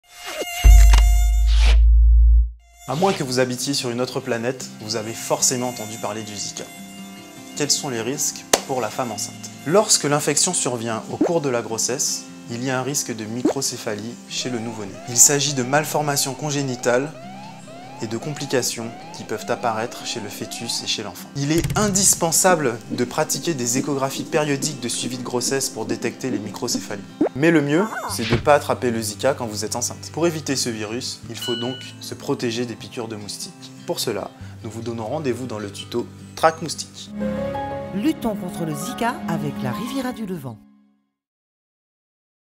Zika - Les risques pour la femme enceinte - Ep3
Le zika, quels sont les risques pour la femme enceinte ?

Luttons contre le zika avec la Riviera du Levant !